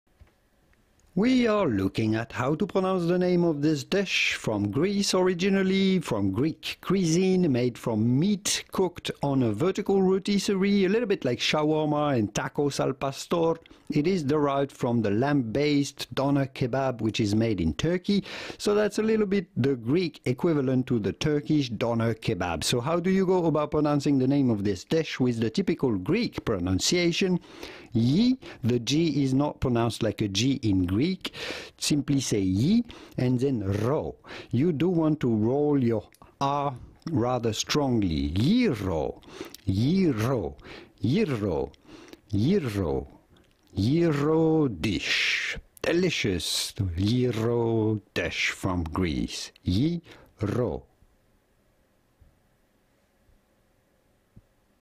How to Pronounce Gyro? (CORRECTLY) Greek Cuisine Pronunciation
Listen and learn how to say Gyro correctly (dish from Greece) with Julien, "how do you pronounce" free pronunciation audio/video tutorials.

Video Content (transcription):
We are looking at how to pronounce the name of this Greek cuisine, made dish made from meat cooked on a vertical rotisserie. Like shawarma and tacos al pastor, it is derived from the lamb-based doner kebab which is made in Turkey. So that's a little bit the Greek equivalent to the Turkish Doner Kebab. So how do you go about pronouncing the name of this dish with the typical Greek pronunciation? YEE. The G is not pronounced like a G in Greek. Simply say YEE. And then RRO. You do want to roll your R rather strongly. Gyro. Gyro. Gyro. Gyro dish. Delicious Gyro dish from Greece. Gyro.

What is a Gyro? A gyro or gyros is a Greek dish made from meat cooked on a vertical rotisserie. Like shawarma and tacos al pastor, it is derived from the lamb-based doner kebab. In Greece it is now most often pork or chicken, whilst beef, chicken, and lamb is common in other countries. Source